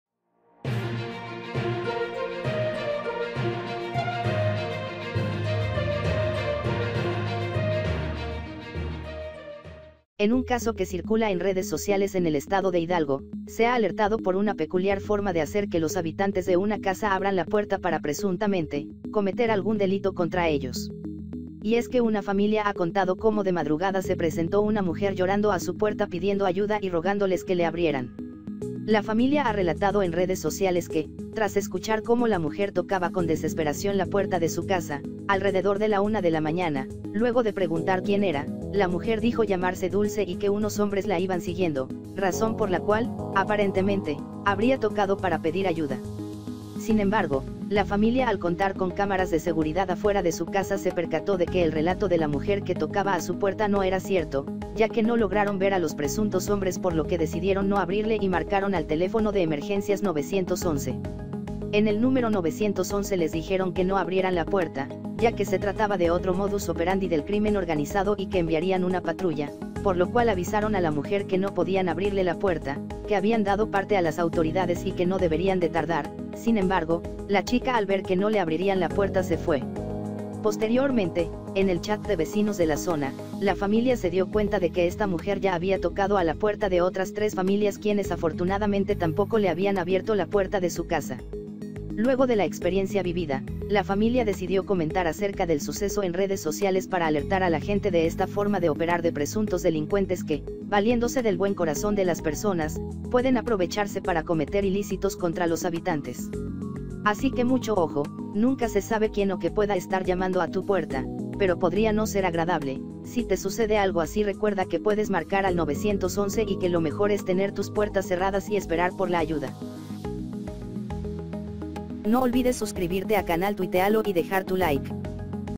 Llorona toca puertas de noche no le abras
En un caso que circula en redes sociales en el estado de Hidalgo, se ha alertado por una peculiar forma de hacer que los habitantes de una casa abran la puerta para presuntamente, cometer algún delito contra ellos. Y es que una familia ha contado cómo de madrugada se presentó una mujer llorando a su puerta pidiendo ayuda y rogándoles que le abrieran.

La familia ha relatado en redes sociales que, tras escuchar cómo la mujer tocaba con desesperación la puerta de su casa, alrededor de la una de la mañana, luego de preguntar quién era, la mujer dijo llamarse Dulce y que unos hombres la iban siguiendo, razón por la cual, aparentemente, habría tocado para pedir ayuda.

Sin embargo, la familia al contar con cámaras de seguridad afuera de su casa se percató de que el relato de la mujer que tocaba a su puerta no era cierto, ya que no lograron ver a los presuntos hombres por lo que decidieron no abrirle y marcaron al teléfono de emergencias 911.

En el número 911 les dijeron que no abrieran la puerta, ya que se trataba de otro modus operandi del crimen organizado y que enviarían una patrulla, por lo cual avisaron a la mujer que no podían abrirle la puerta, que habían dado parte a las autoridades y que no deberían de tardar; sin embargo, la chica al ver que no le abrirían la puerta se fue.

Posteriormente, en el chat de vecinos de la zona, la familia se dio cuenta de que esta mujer ya había tocado a la puerta de otras tres familias quienes afortunadamente tampoco le habían abierto la puerta de su casa.

Luego de la experiencia vivida, la familia decidió comentar acerca del suceso en redes sociales para alertar a la gente de esta forma de operar de presuntos delincuentes que, valiéndose del buen corazón de las personas, pueden aprovecharse para cometer ilícitos contra los habitantes.

Así que mucho ojo, nunca se sabe quién o qué pueda estar llamando a tu puerta, pero podría no ser agradable, si te sucede algo así recuerda que puedes marcar al 911 y que lo mejor es tener tus puertas cerradas y esperar por la ayuda.